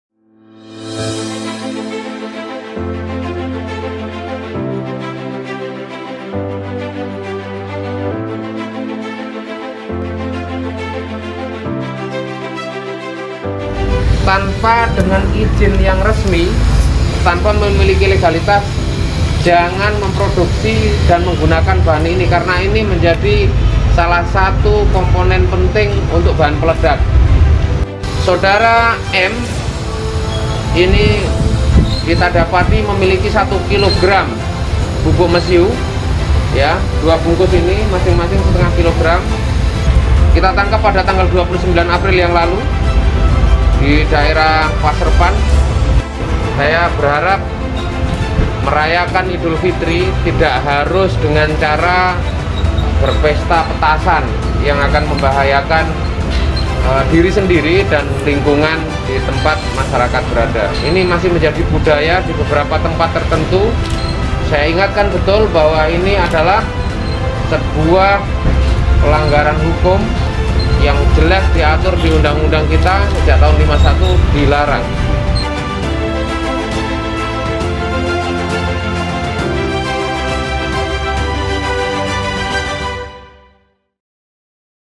JELANG HARI RAYA IDUL FITRI PRIA INI DITANGKAP POLISI KARENA NYAMBI JUALAN SERBUK BAHAN PELEDAK.
JANGAN COBA-COBA !!! JELANG PERAYAAN HARI RAYA IDUL FITRI PRIA INI DITANGKAP POLISI KARENA NYAMBI JUALAN SERBUK BAHAN PELEDAK.

Masyarakat yang ingin merayakan lebaran dengan pesta petasan ada baiknya mengurungkan niatnya. Kasus yang menimpa Manan (67) berikut bisa dijadikan pelajaran.

Warga Desa Ampelsari, Kecamatan Pasrepan, Kabupaten Pasuruan ini ditahan polisi pada Kamis, 29 April 2021, usai kedapatan menjual serbuk bahan peledak di rumahnya, Manan beserta serbuk bahan peledak miliknya, tadi siang, dibeber dihadapan sejumlah wartawan di Mapolres Pasuruan, Selasa (04/05/2021).

“Kalau di Pasrepan dulu ada dua rumah yang ambruk gara-gara ledakan, ya bahan seperti ini lah yang meledak. Kalau dalam jumlah besar, gedung pun bisa rontok,” ujar Kapolres Pasuruan AKBP Rofiq Ripto Himawan didampingi AKP Adrian Wimbarda Kasatreskrim beserta para PJU Polres Pasuruan

Kepada masyarakat luas, ia memperingatkan untuk tidak main-main dengan petasan. Terutama saat merayakan Idul Fitri.

“Ini masih menjadi budaya di beberapa tempat tertentu. Saya ingatkan betul bahwa ini adalah sebuah pelanggaran hukum yang jelas diatur dalam undang-undang kita sejak tahun 1951. Tolong masyarakat hindari perayaan Idul Fitri dengan cara menyulut maupun membuat petasan,” paparnya.

Sementara itu ditempat yang sama, Manan bersikukuh tidak mengenal si penjual sayur keliling. Dirinya mengaku hanya menjalankan titipan si penjual tersebut. kasus ini masih kita dalami. Karena pelaku kurang kooperatif.

“Orangnya ngaku dari Kluwut. Tapi saya enggak kenal. Rumah orangnya saya enggak tahu. Saya cuma dititipi, disuruh jualkan. Disuruh jual 300 ribu perkilo. Sebelumnya saya juga belum pernah jualan barang seperti ini,” akunya.